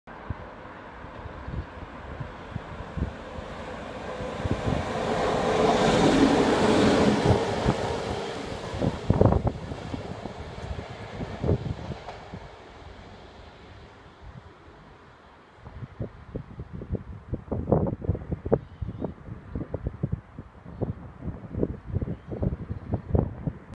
Colas Training-60076
Colas Rail Class 60 No. 60076 is seen here passing through Lostock Hall station, returning to Doncaster on a driver training run. This is in preparation for the Preston Docks to Linsey Bitumen contract moving to a Colas working.